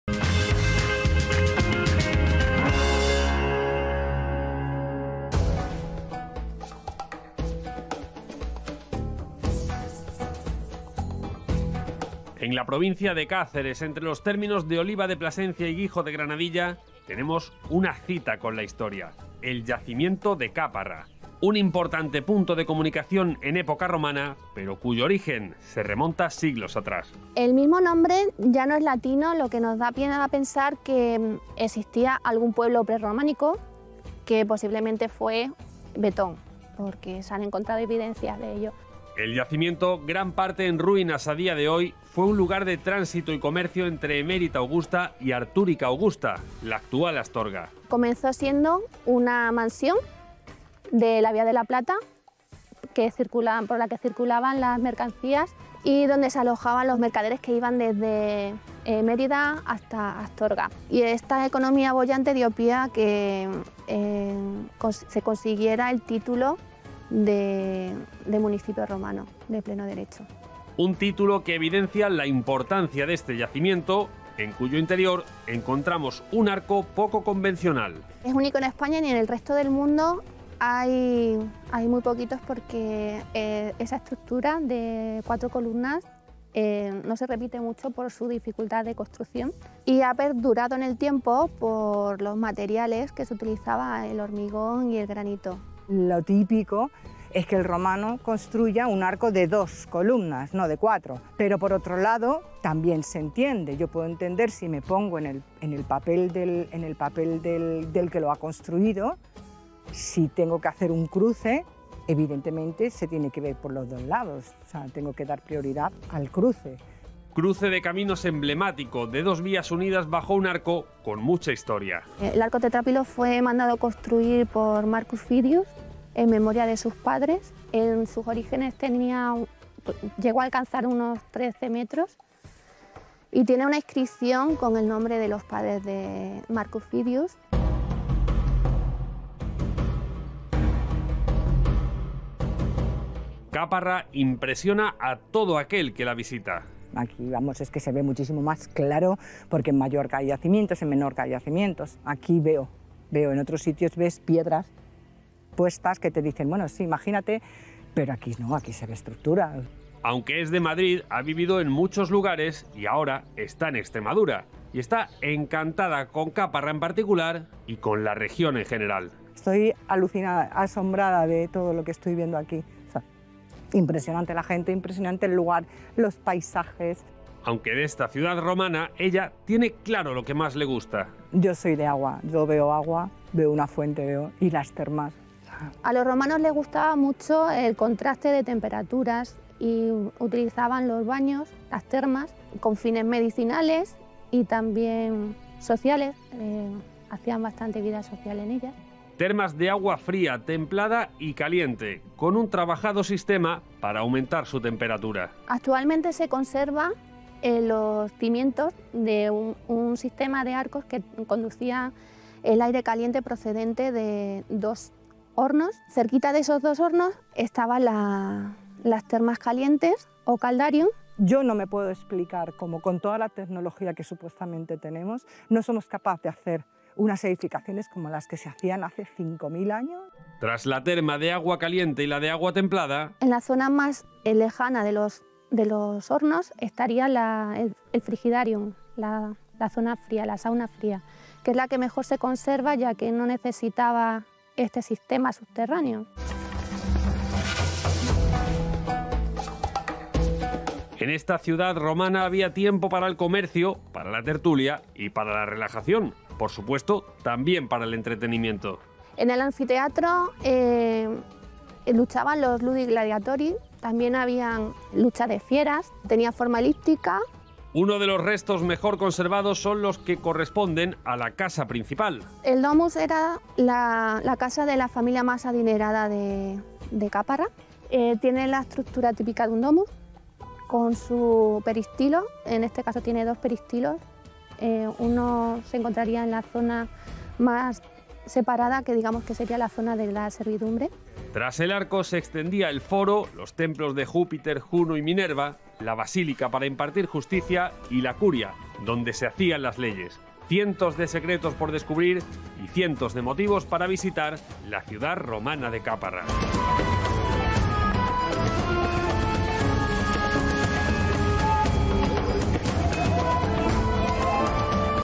Un arco único en la Península Ibérica | Territorio Extremadura
La ciudad romana de Cáparra, entre Oliva de Plasencia y Guijo de Granadilla, es un yacimiento único en España gracias, principalmente, a su arco. Es una de las estructuras que mejor se conserva de este emplazamiento, que también contaba con termas, anfiteatro y foro, y que fue un importante cruce de caminos entre las actuales Mérida y Astorga.